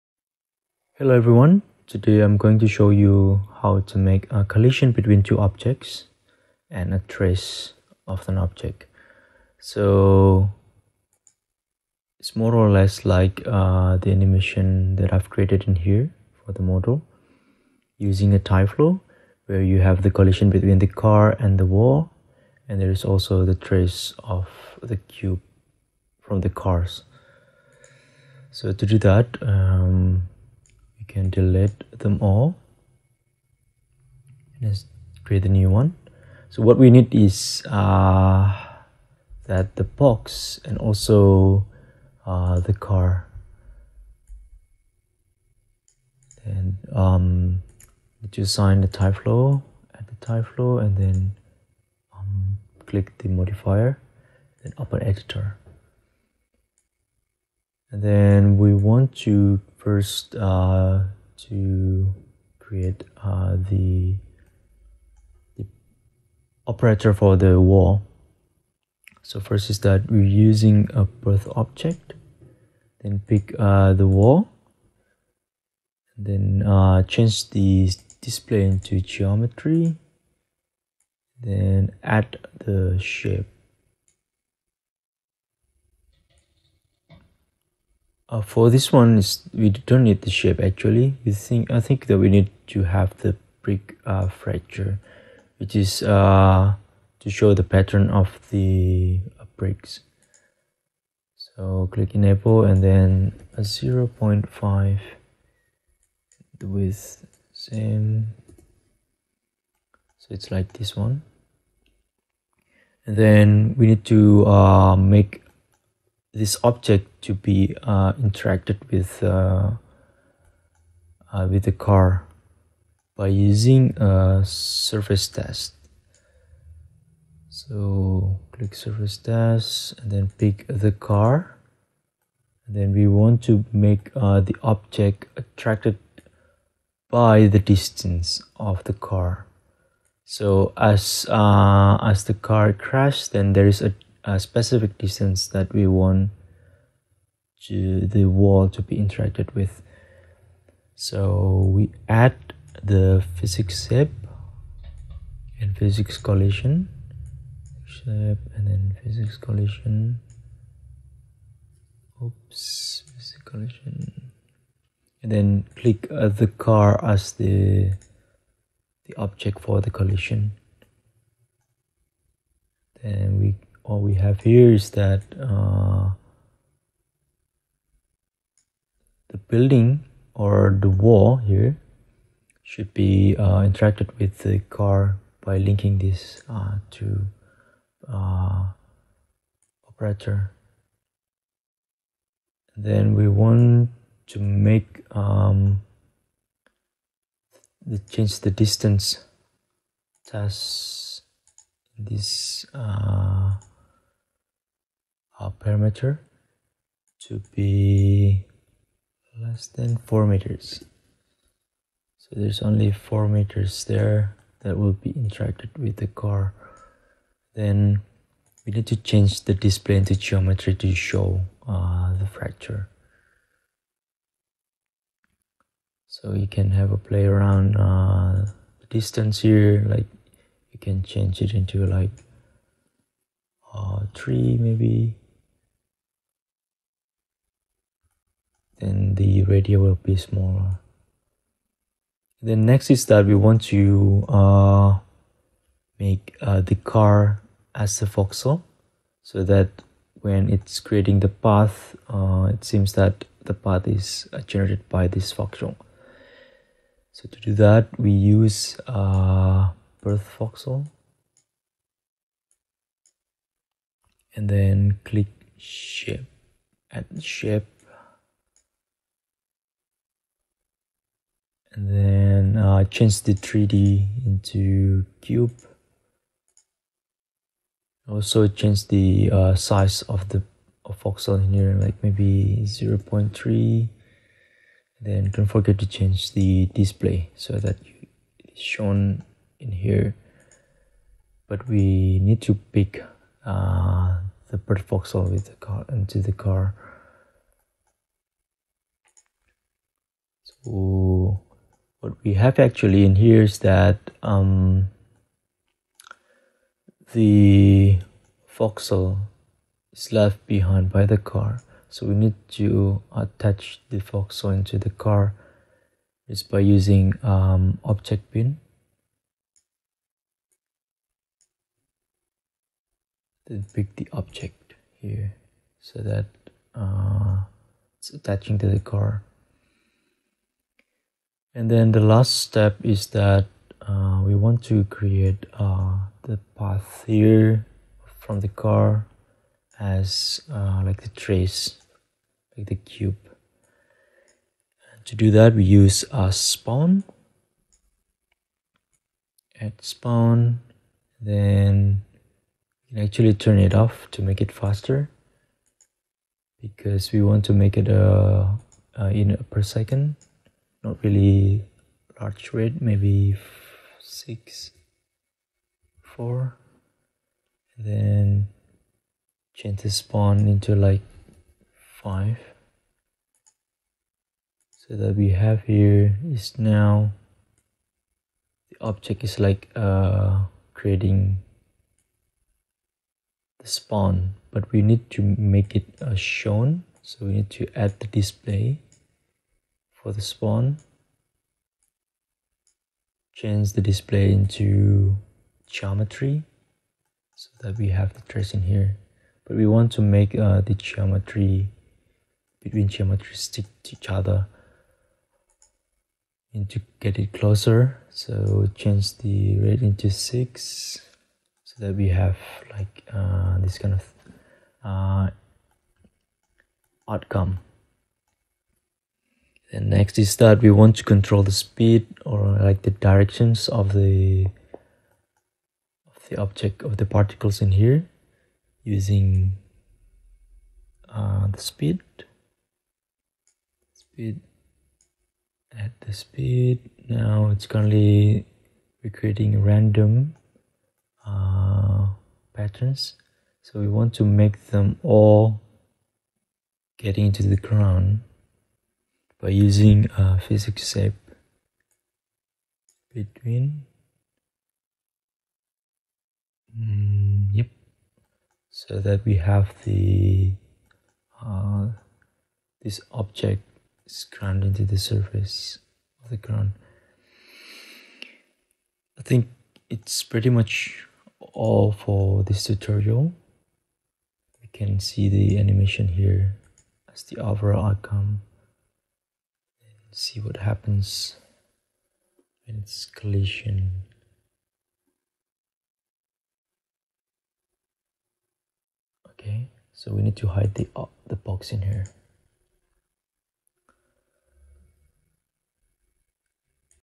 2019FUM Fadhil _ 3ds Max : Tyflow Collision & Spawn
The collision of the brick: tyflow brick fracture and Physx collision
The spawning cubes: tyflow voxels, object bind and spawn.